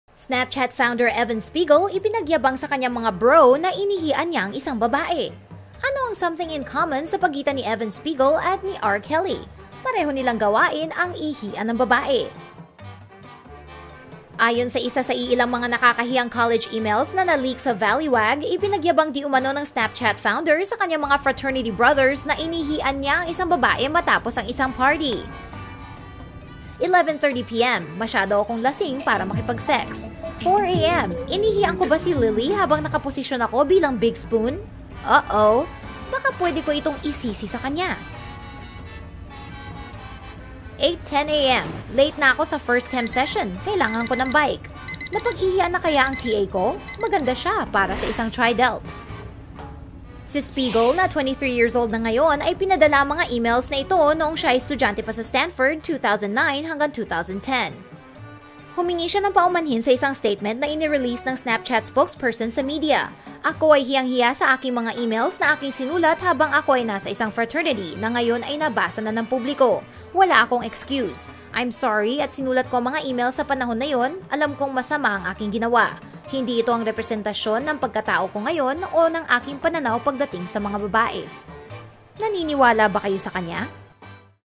Snapchat founder Evan Spiegel, inihian ang isang babae sa college (atbp nakakahiyang detalye!)
Snapchat founder Evan Spiegel, inihian ang isang babae sa college (at iba pang nakakahiyang detalye)!

Snapchat founder Evan Spiegel, ipinagyabang sakanyang mga 'bro' na inihian niya ang isang babae!

Ano ang something in common sa pagitan ni Evan Spiegel, at ni R. Kelly?

Pareho nilang gawain ang ihian ang babae!

Ayon sa isa sa iilang mga nakakahiyang college emails na na-leak sa Valleywag, ipinagyabang diumano ng Snapchat founder sa kanyang mga fraternity brothers na inihian niya ang isang babae matapos ang isang party.

"11:30pm. Masyado akong lasing para makipag-sex."
"4am. Inihian ko ba si Lyly habang naka-posisyon ako bilang big spoon?"
"Uh-oh."
"Baka pwede koi tong isisi sa kanya."
"8:10am. Late nako sa first chem session. Kailangan ko ng bike. Napag-ihian na kaya ang TA ko? Maganda siya, para sa isang TriDelt."

Si Spiegel, na 23 years old na ngayon, ay pinadala ang mga emails na ito noong siya ay estudyante pa sa Stanford, 2009 hanggang 2010.

Humingi siya ng paumanhin sa isang statement na ini-release ng Snapchat spokesperson sa media: "Ako ay hiyang-hiya sa aking mga emails, na aking sinulat habang ako ay nasa isang fraternity, na ngayon ay nabasa na ng publiko. Wala akong excuse. I'm sorry at sinulat ko ang mga emails sa panahon na iyon, alam kong masama ang aking ginawa. Hindi ito ang representasyon ng pagkatao ko ngayon, o ang aking pananaw pagdating sa mga kababaihan."